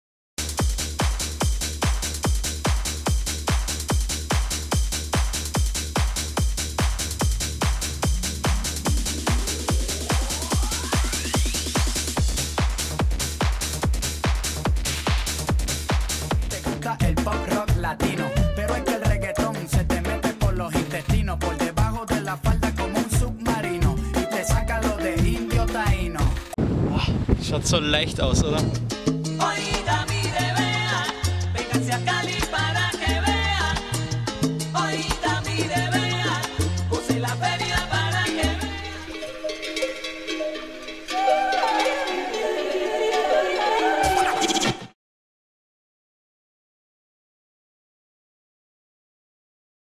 el yaque kiting short version
Kurzes kite video um die You Tube qualität zu testen bzw. zu optimieren